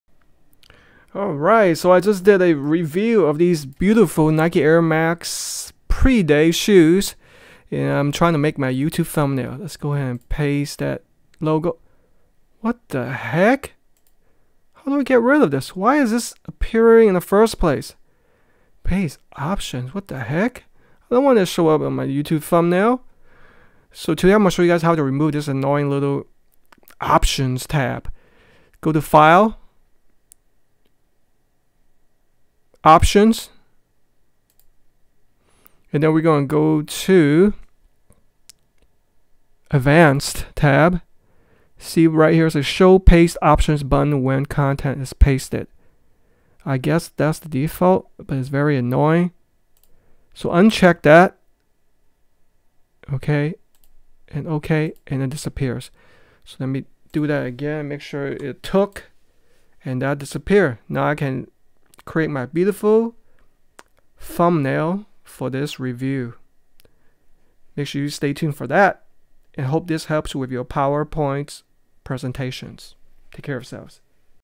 How To Hide Paste Options Button In Microsoft PowerPoint
Tired of the annoying paste options button appearing in your PowerPoint slides?